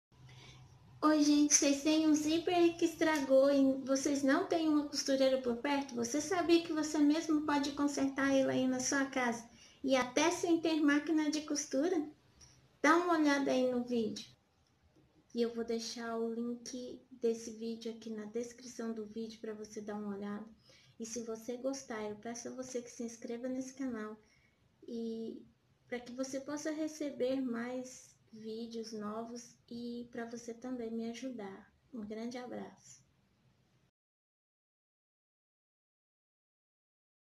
Você tem um zíper que estragou?
Você tem um zíper que estragou e não tem uma costureira por perto? Você sabia que pode consertar esse zíper sem ter máquina de costura?